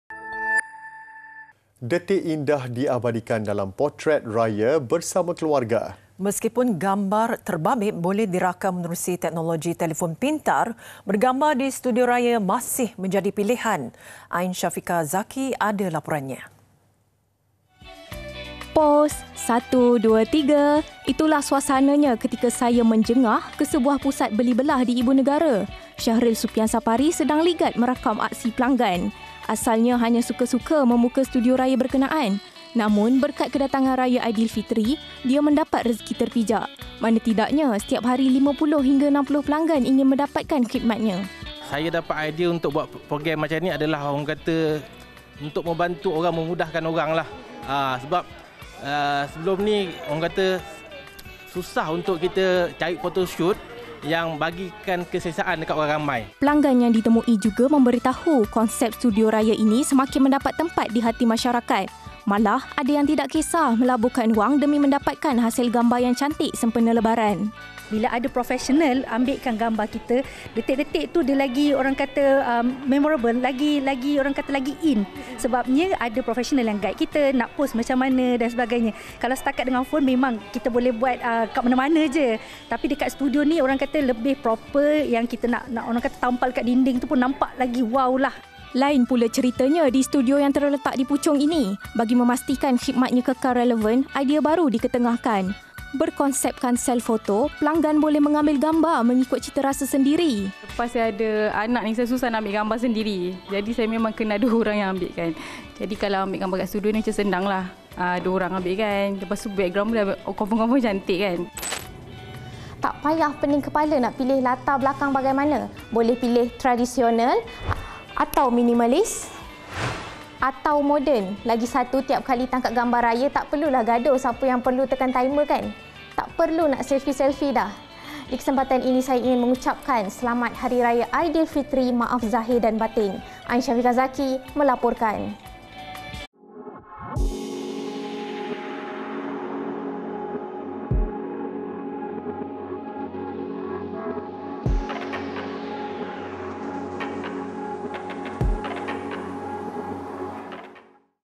AIDILFITRI | Rakam Memori Lebaran Di Studio Raya
Detik indah diabadikan dalam potret raya bersama keluarga.

Meskipun gambar terbabit boleh dirakam menerusi teknologi telefon pintar, bergambar di studio raya masih menjadi pilihan.

Wartawan Ain Shafiqah Zaki ada laporannya.